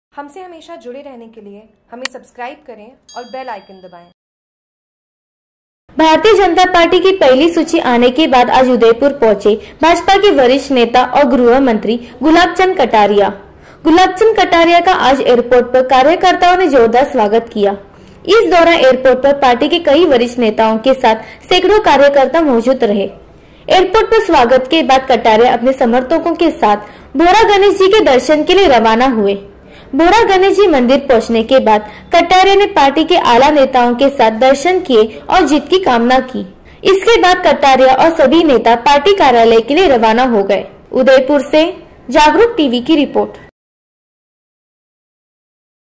JAGRUK TV : उदयपुर : कटारिया का जोरदार स्वागत
भारतीय जनता पार्टी की पहली सूची आने के बाद आज उदयपुर पहुंचे भाजपा के वरिष्ठ नेता और गृहमंत्री गुलाबचंद कटारिया का आज एयरपोर्ट पर कार्यकर्ताओं ने जोरदार स्वागत किया। इस दौरान एयरपोर्ट पर पार्टी के कई वरिष्ठ नेताओं के साथ साथ सैकड़ों कार्यकर्ता मौजूद रहे एयरपोर्ट पर स्वागत के बाद कटारिया अपने समर्थकों के साथ साथ बोहरा गणेश जी के दर्शनों के लिए रवाना हुए। बोहरा गणेश जी मंदिर पहुंचने के बाद कटारिया ने पार्टी के आला नेताओं के साथ दर्शन किए और जीत की कामना की। इसके बाद कटारिया और सभी नेता पार्टी कार्यालय के लिए रवाना हो गए।